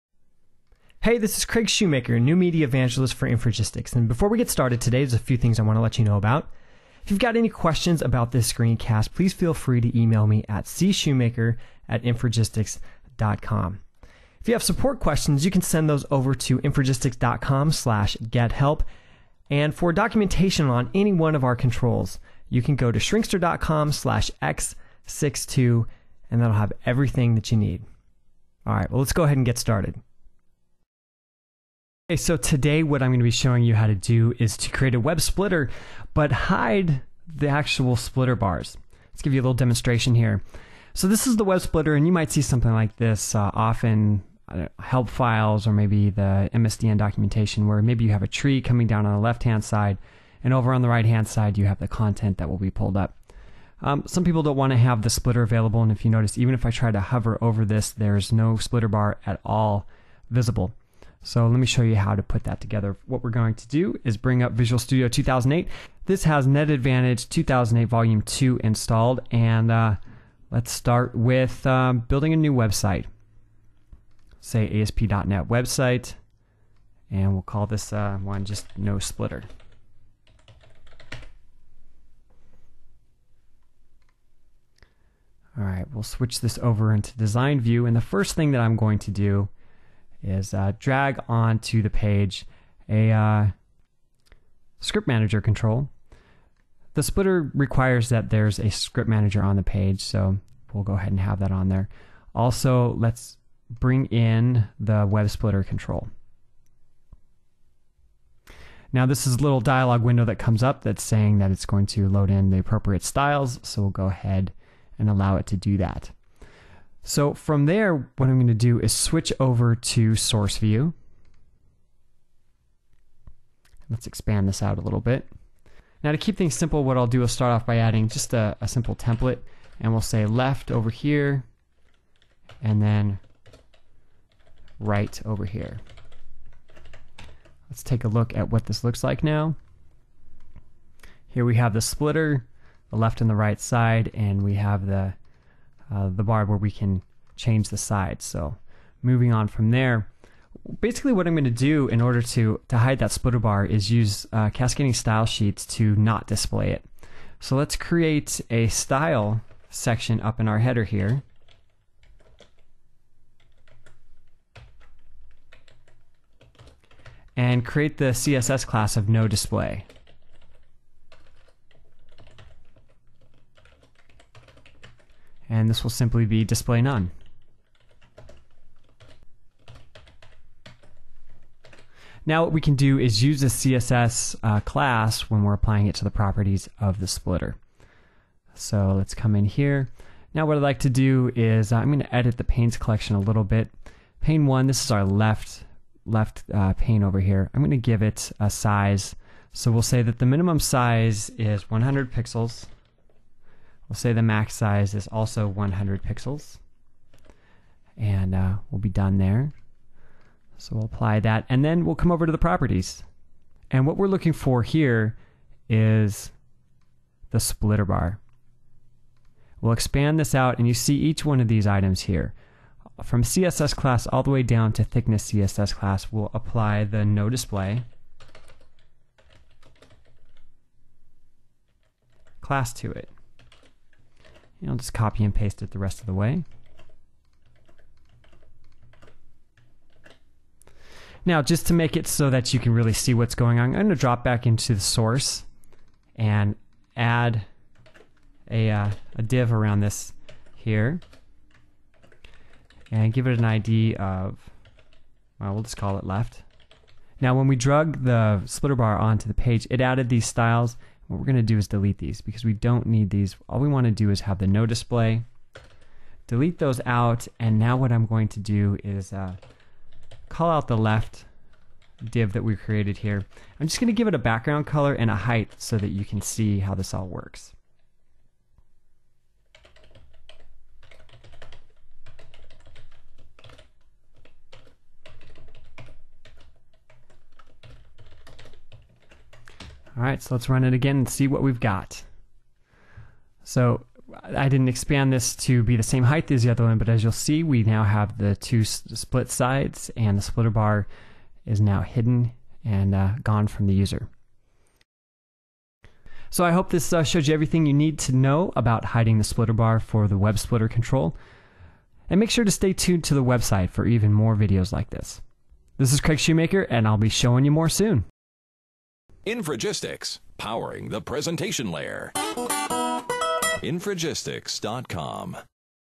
Hiding WebSplitter Splitter Bar
Demonstrates how to hide the splitter bar when using NetAdvantage for ASP.NET WebSplitter control